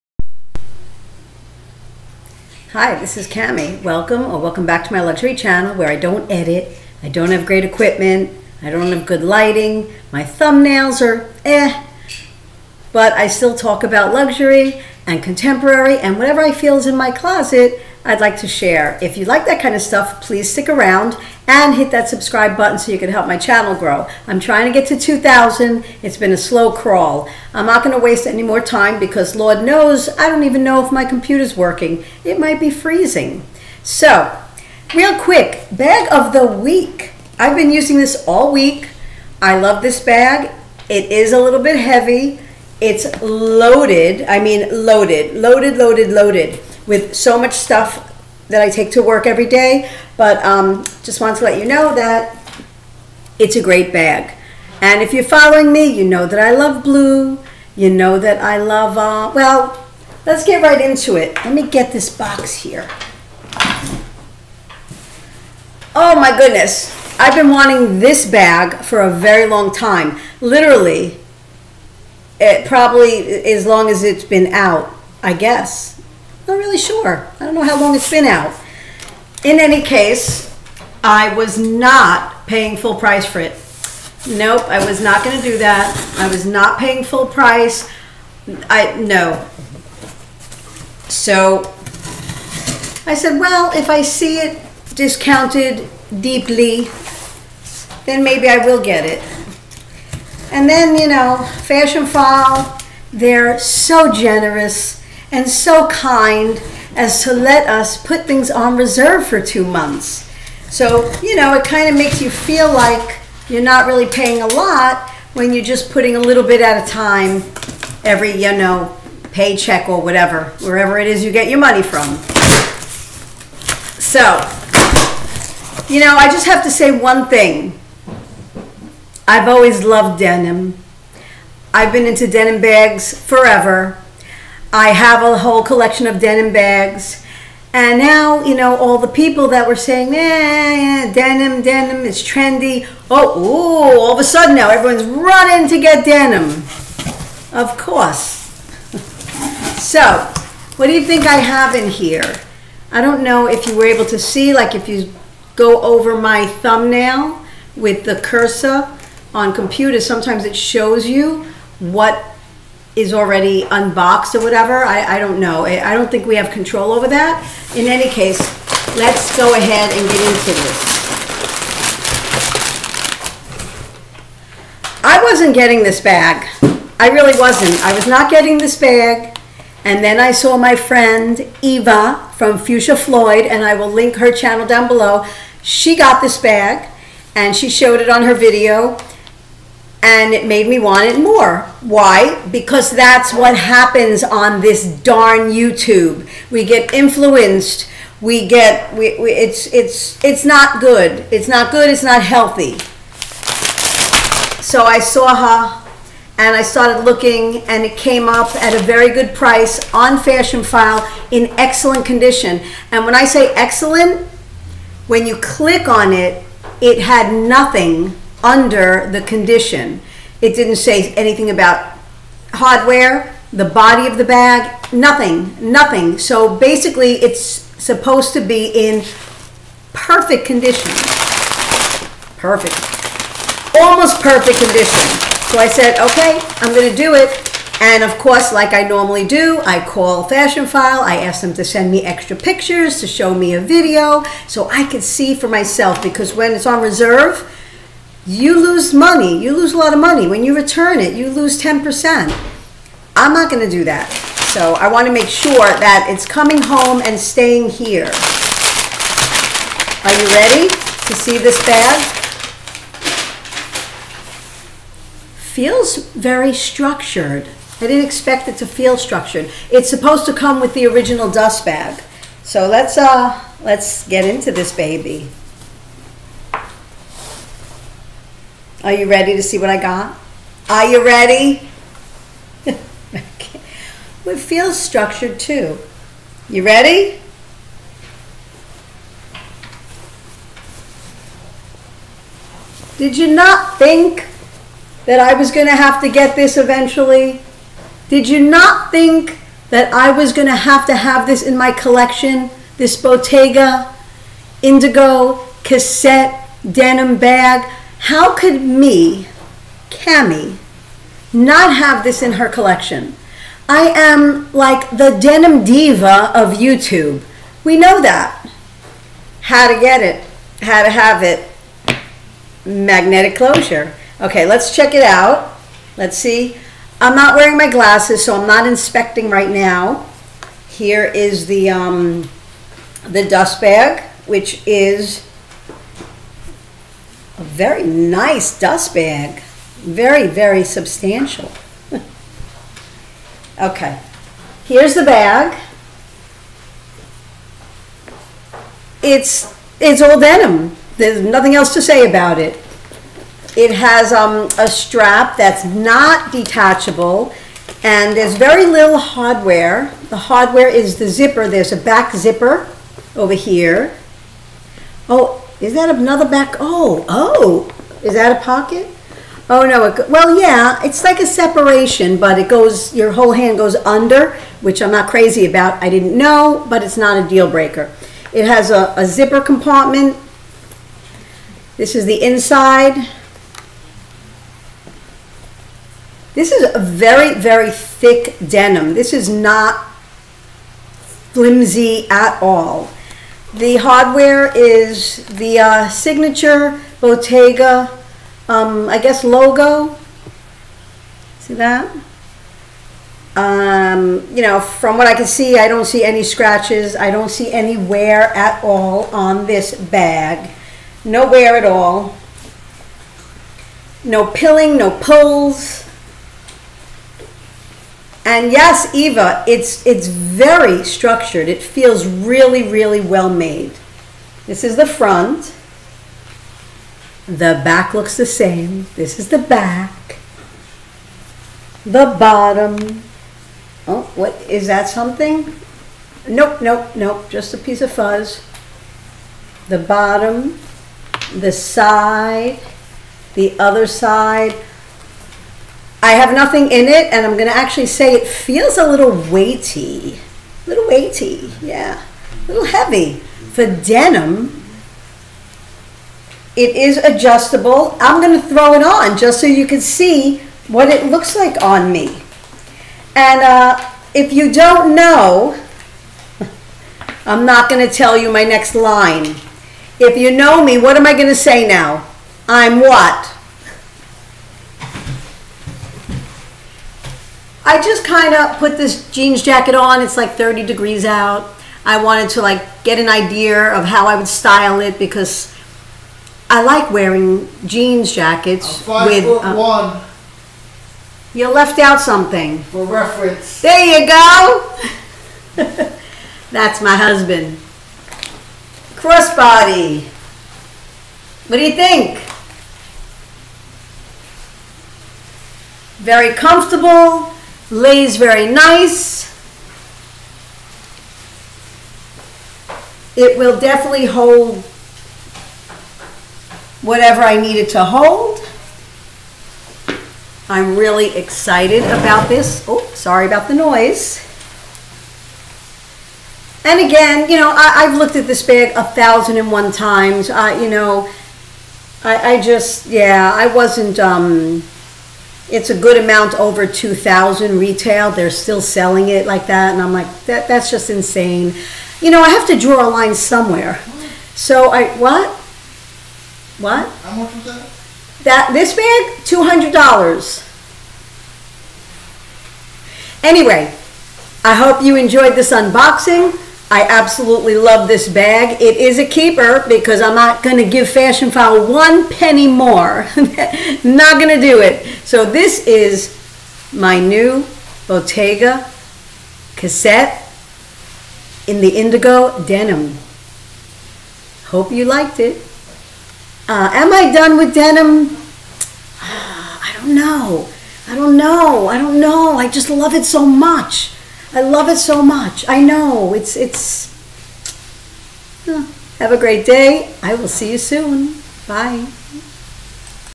LUXURY DENIM HANDBAG UNBOXING | HUSBAND FREAKS OUT 😬WAIT UNTIL THE END
How many denim handbags can one girl have?